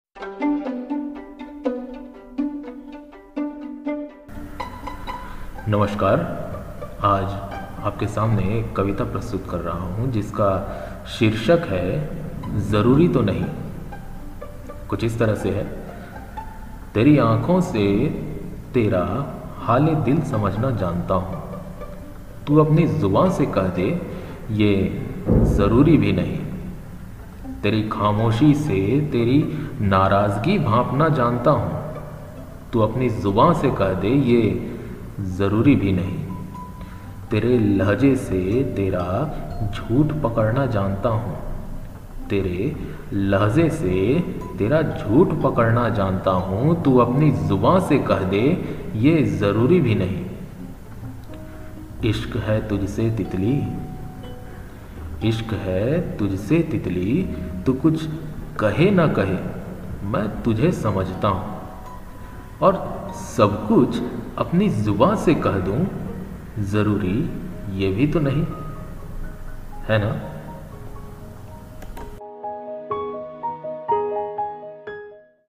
ज़रूरी तो नहीं
हर बात कह देना जरूरी तो नहीं क्योंकि कभी कभी जज़्बातों को समझने में जो आनंद है वह कह देने में नहीं ।